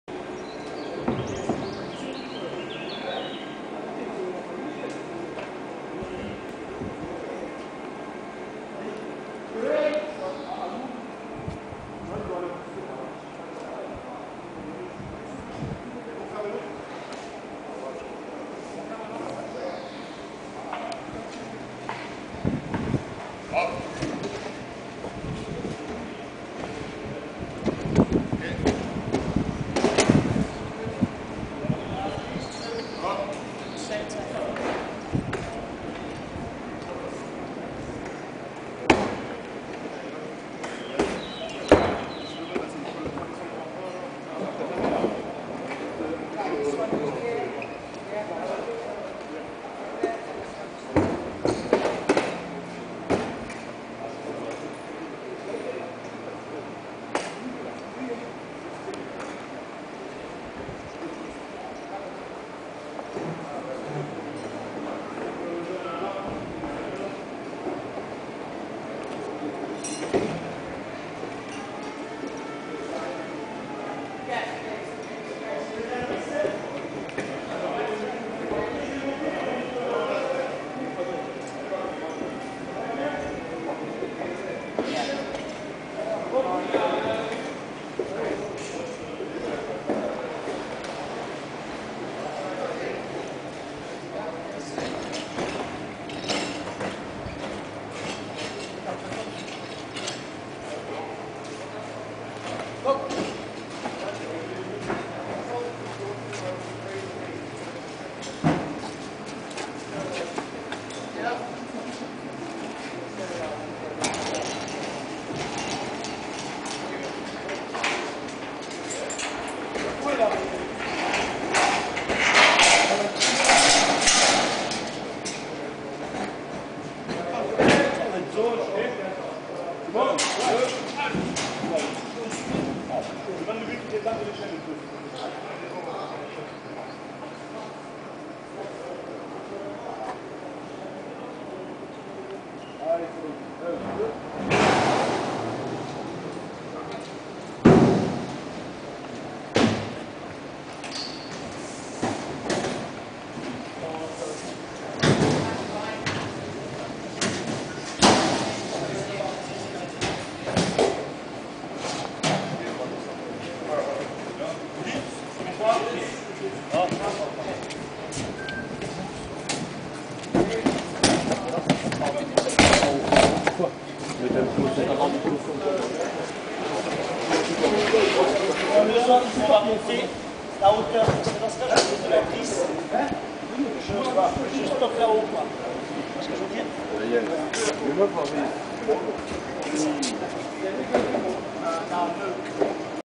On stage with Robert Plants Crew in a castle
Watch in delight as the merry little Robert Plant crew set up for the gig in Carcassonne. Watch in wonder and awe as the gear has to been craned over the walls, because there ain't no way in for the truck...!